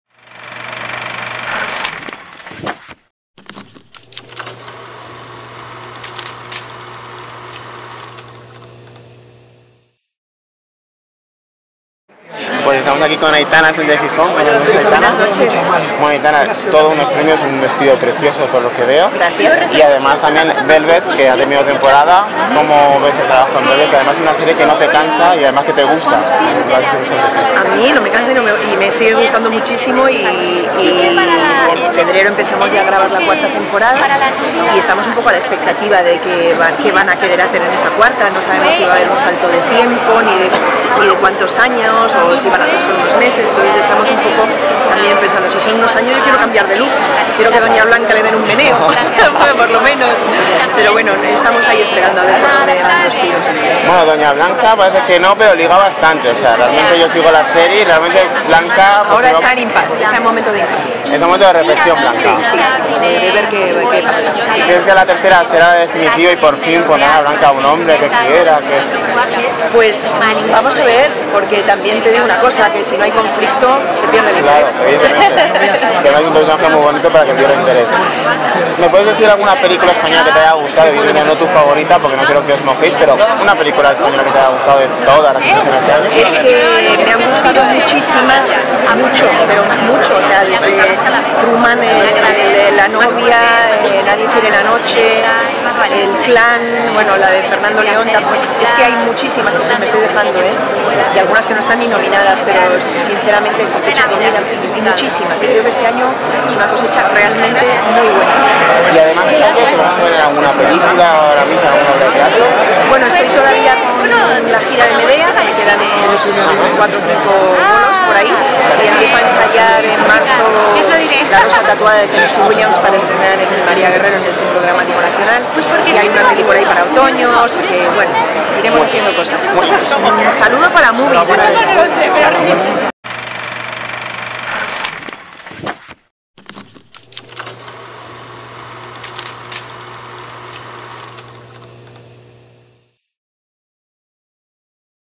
Aitana Sánchez Gijón
Entrevista con la actriz Aitana Sánchez Gijón en los Premio Forqué